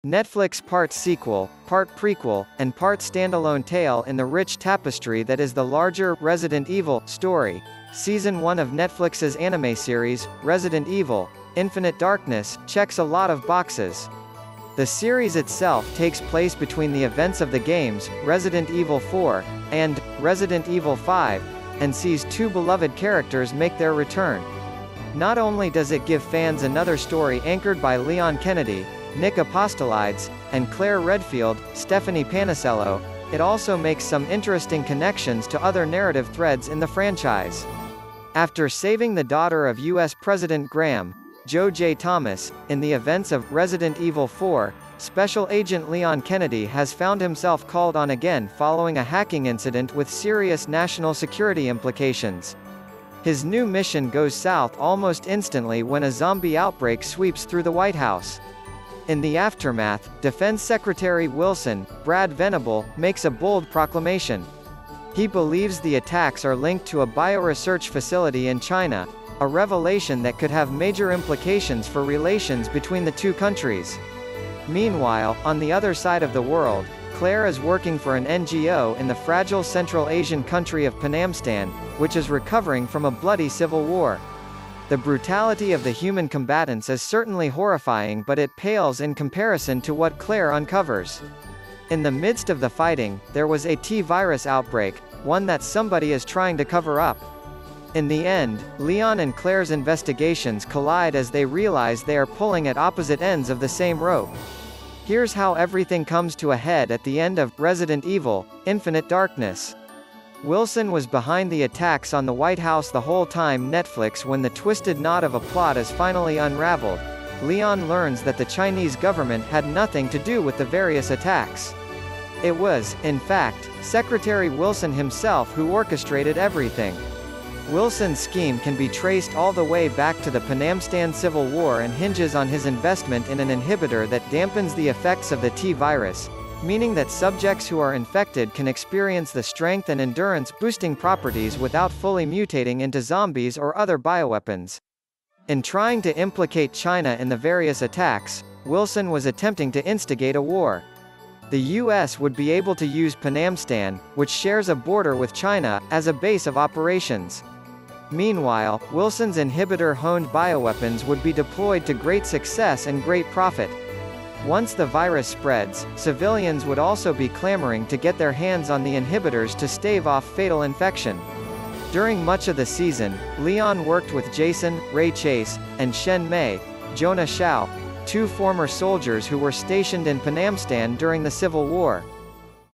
The Ending Of Resident Evil Infinite Darkness Season 1 Explained
NetflixPart sequel, part prequel, and part stand-alone tale in the rich tapestry that is the larger "Resident Evil" story, Season 1 of Netflix's anime series "Resident Evil: Infinite Darkness" checks a lot of boxes. The series itself takes place between the events of the games "Resident Evil 4" and "Resident Evil 5" and sees two beloved characters make their return. Not only does it give fans another story anchored by Leon Kennedy (Nick Apostolides) and Claire Redfield (Stephanie Panisello), it also makes some interesting connections to other narrative threads in the franchise. After saving the daughter of US President Graham (Joe J. Thomas) in the events of "Resident Evil 4," Special Agent Leon Kennedy has found himself called on again following a hacking incident with serious national security implications. His new mission goes south almost instantly when a zombie outbreak sweeps through the White House. In the aftermath, Defense Secretary Wilson (Brad Venable) makes a bold proclamation. He believes the attacks are linked to a bioresearch facility in China, a revelation that could have major implications for relations between the two countries. Meanwhile, on the other side of the world, Claire is working for an NGO in the fragile central-Asian country of Penamstan, which is recovering from a bloody civil war. The brutality of the human combatants is certainly horrifying but it pales in comparison to what Claire uncovers. In the midst of the fighting, there was a T-Virus outbreak, one that somebody is trying to cover up. In the end, Leon and Claire's investigations collide as they realize they are pulling at opposite ends of the same rope. Here's how everything comes to a head at the end of "Resident Evil: Infinite Darkness."Wilson was behind the attacks on the White House the whole timeNetflixWhen the twisted knot of a plot is finally unraveled, Leon learns that the Chinese government had nothing to do with the various attacks. It was, in fact, Secretary Wilson himself who orchestrated everything. Wilson's scheme can be traced all the way back to the Penamstan Civil War and hinges on his investment in an inhibitor that dampens the effects of the T-Virus, meaning that subjects who are infected can experience the strength and endurance-boosting properties without fully mutating into zombies or other bioweapons. In trying to implicate China in the various attacks, Wilson was attempting to instigate a war. The US would be able to use Penamstan, which shares a border with China, as a base of operations. Meanwhile, Wilson's inhibitor-honed bioweapons would be deployed to great success and great profit. Once the virus spreads, civilians would also be clamoring to get their hands on the inhibitors to stave off fatal infection. During much of the season, Leon worked with Jason (Ray Chase) and Shen May (Jona Xiao), two former soldiers who were stationed in Penamstan during the civil war.